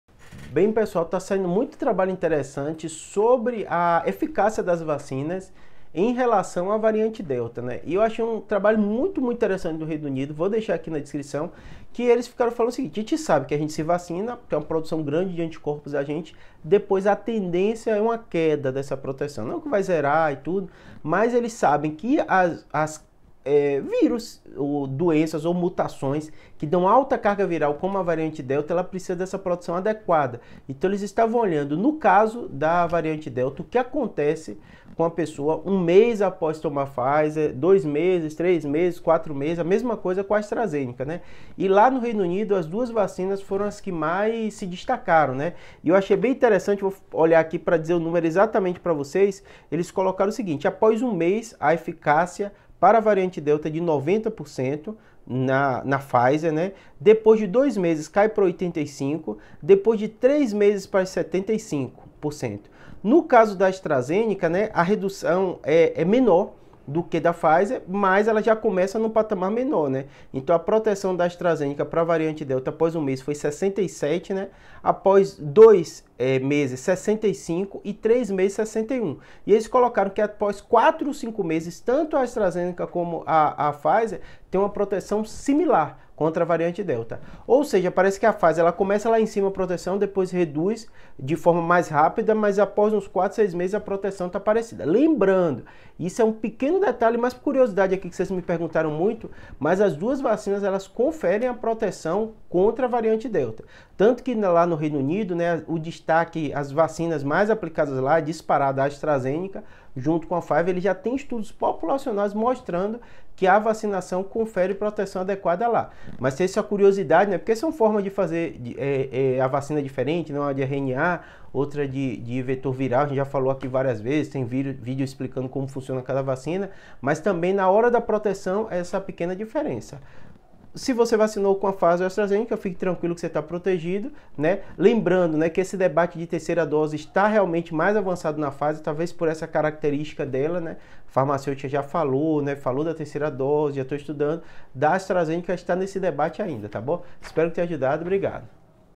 VACINAS SÃO EFICAZES NA VARIANTE DELTA ?
COVID vaccines protect against Delta, but their effectiveness wanes
Researchers at the University of Oxford, UK, and the country’s Office for National Statistics analysed a vast data set comprising the results of 2,580,021 PCR tests to check for SARS-CoV-2 from 384,543 UK adults between 1 December 2020 and 16 May 2021 — when the Alpha variant was dominant — and 811,624 test results from 358,983 people between 17 May and 1 August 2021, when the Delta variant was more prevalent.